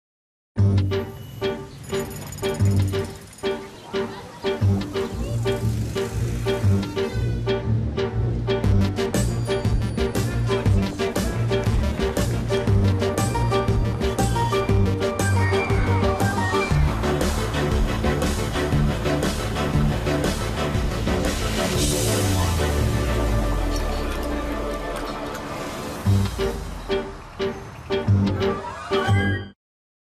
Bullring, Birmingham "We are So City" TV ad
The brand new Bullring TV ad, from the makers of Wallace and Gromit.